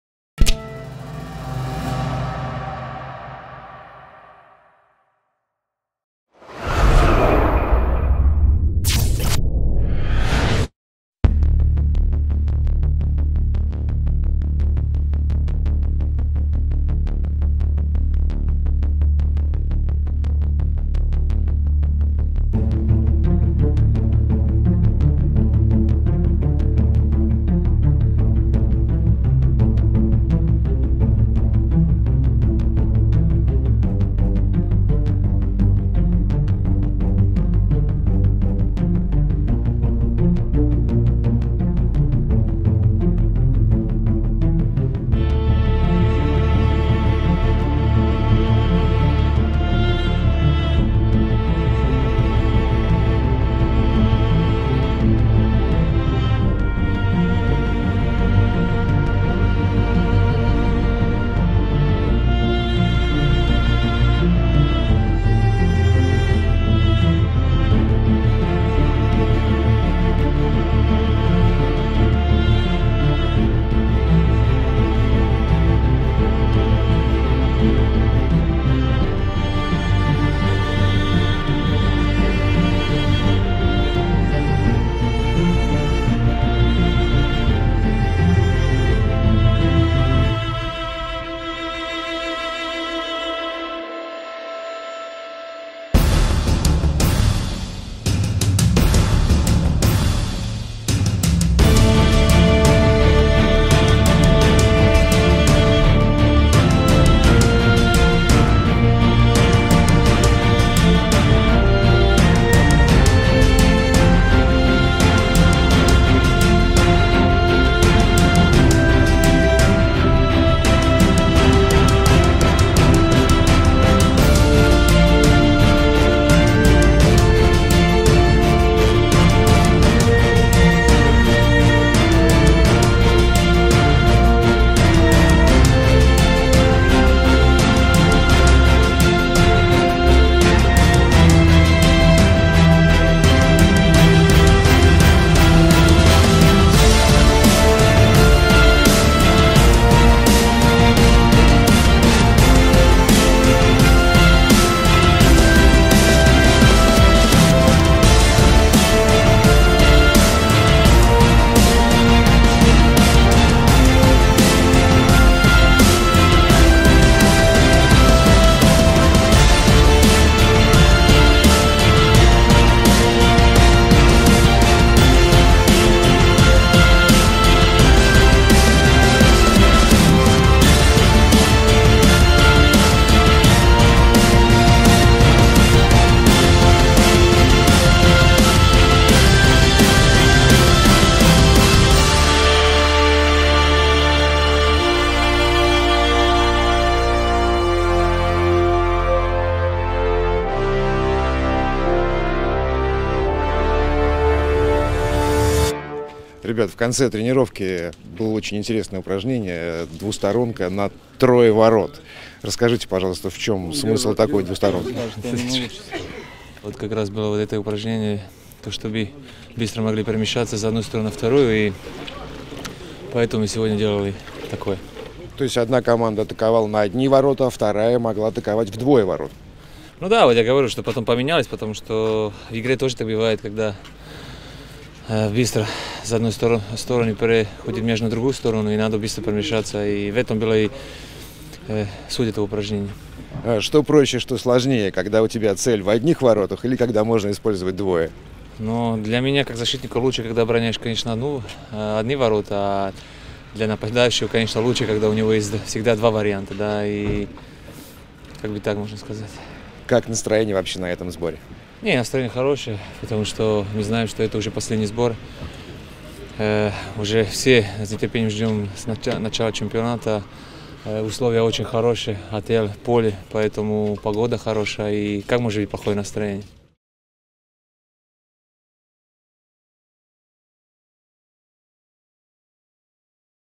«С нетерпением ждем возобновления чемпионата!»
Работа вратарей, необычная «двухсторонка» на трое ворот и комментарий Яна Дюрицы – в яркой зарисовке от клубного телевидения. «Локо-ТВ».